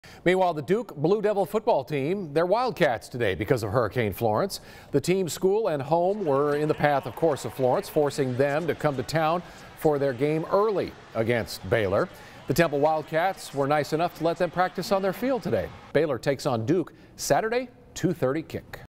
Duke football practices at Temple Stadium
Hurricane Florence forced the team to come to town early for their game against Baylor.